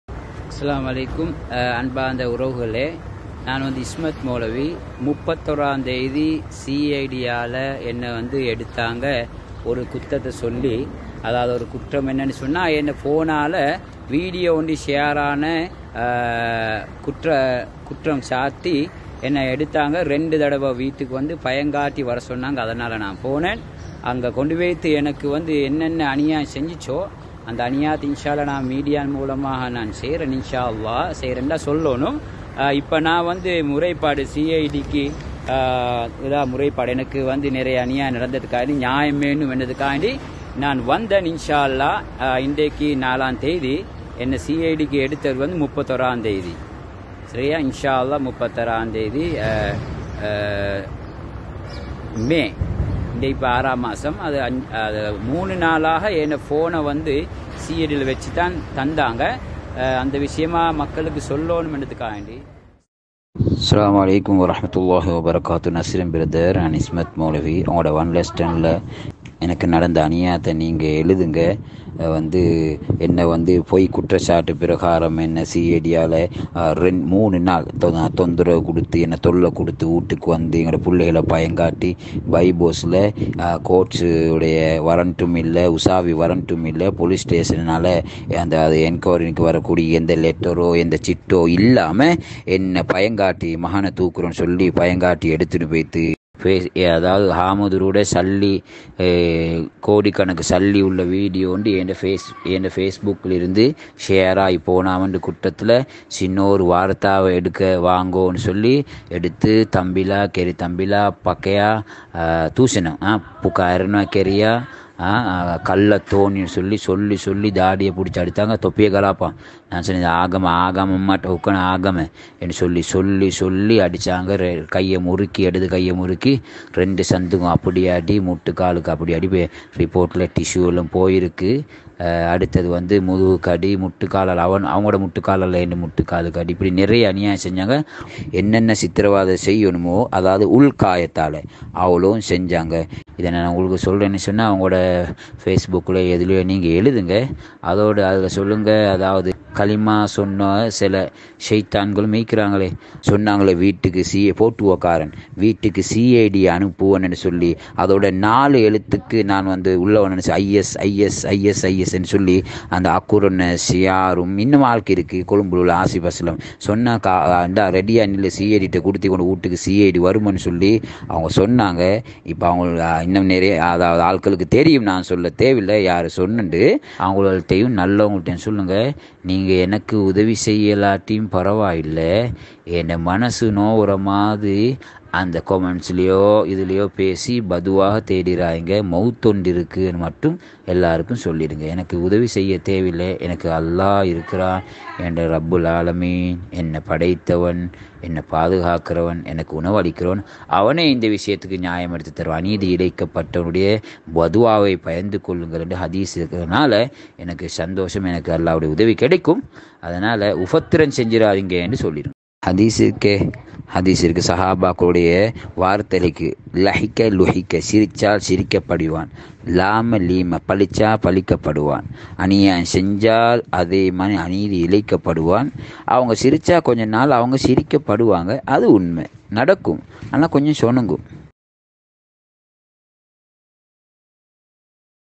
Cid யால் இஸ்மத் மெவலவி அநியாயமாக தாக்கப்பட்டார் அல்லாவின் உதவியை மட்டும் எதிர்பார்கின்றேன் யா அல்லா
Video from Ismath Moulavi